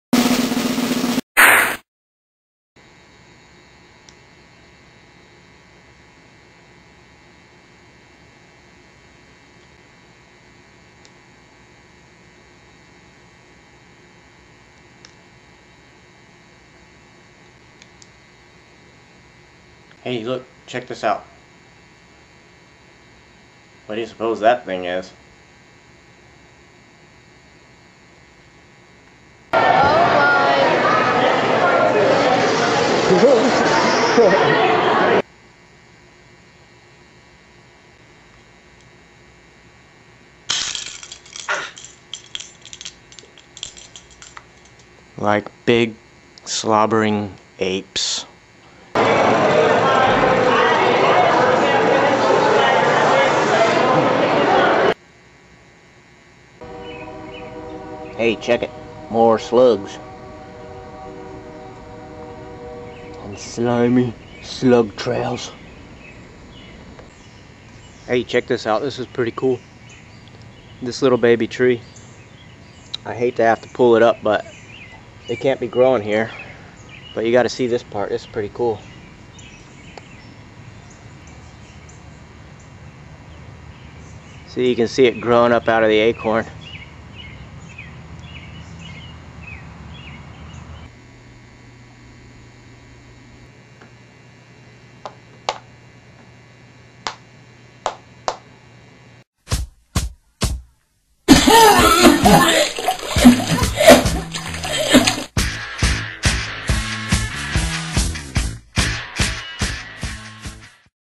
Some Random Clips
Every so often I put together clips of stuff what never made it into other videos, this is not one of those times...I mean, wait!  Yes it is, I took random clips an made a whole new video because my camera's broken...okay, thas a lie, it ain't broken, per se...I jus had too much yard work to do, since it finally ain't rainin, so I didn't leave myself any time to do any of the fancy, important type things I usually do...like my rubbery face video, or my venting video, or anything involving full frontal nudity or magic lamps what got mystical beings enshrined in them or anything what requires CGI or big city lawyers.

Hey, that clip worked great for the thumbnail...an I didn't even plan it, it's like destiny or something...ever know a girl named Destiny?  I haven't.